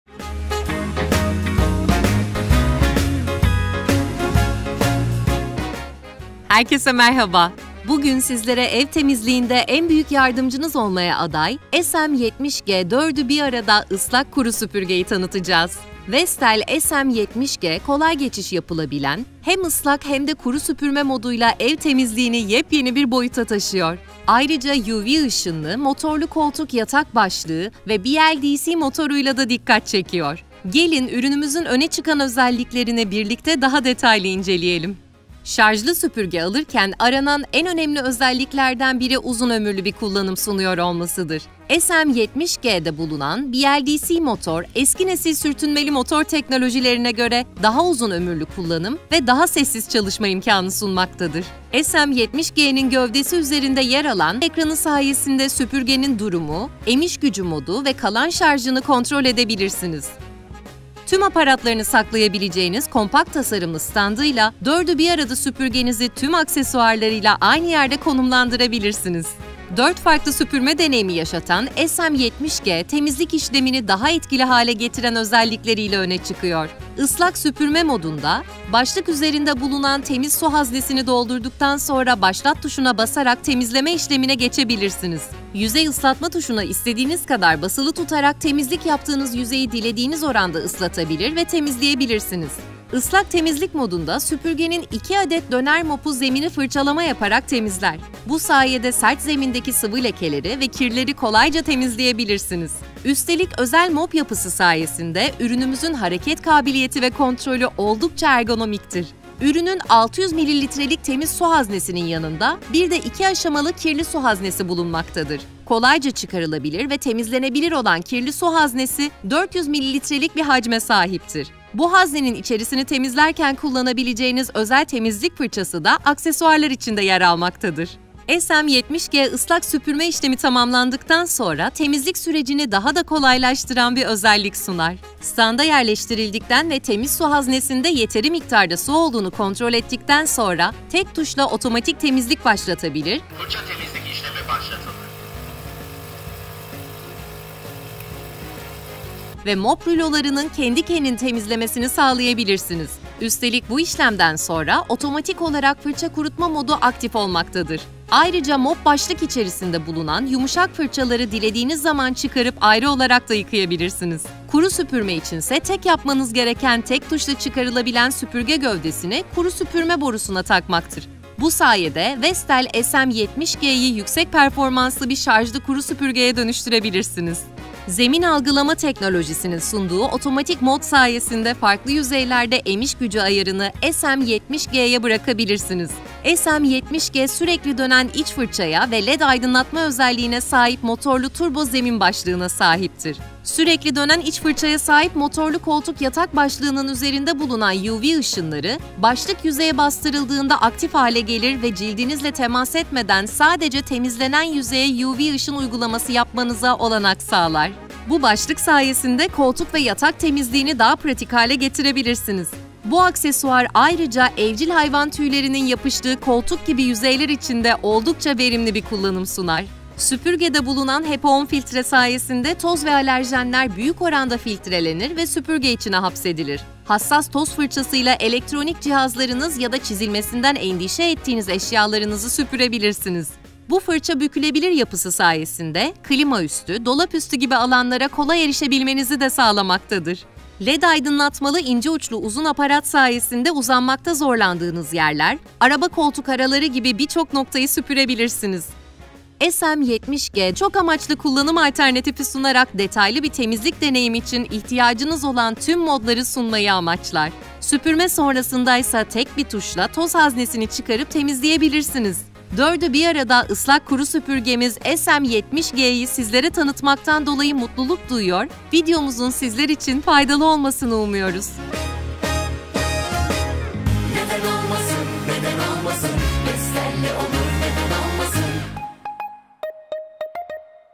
Temizlikte her ihtiyaca tek cevap: Vestel SM70 G
Islak süpürme, kuru süpürme, mop ve el süpürgesiyle temizlik Vestel SM70 G'yle evinizde.